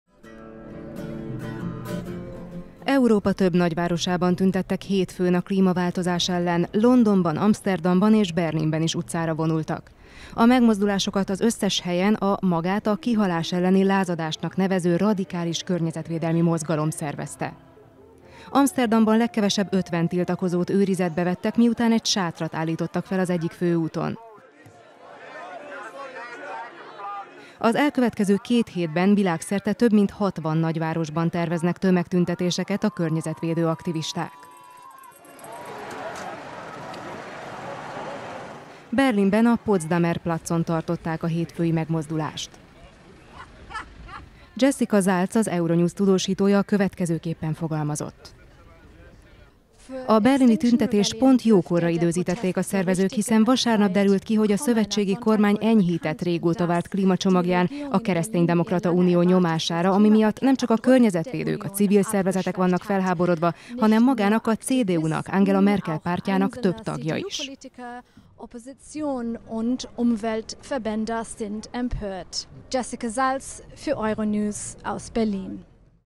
Extinction Rebellion: tüntetőket vettek őrizetbe Amszterdamban
Az elkövetkező két hétben világszerte több mint 60 nagyvárosban terveznek tömegtüntetéseket a környezetvédő aktivisták. …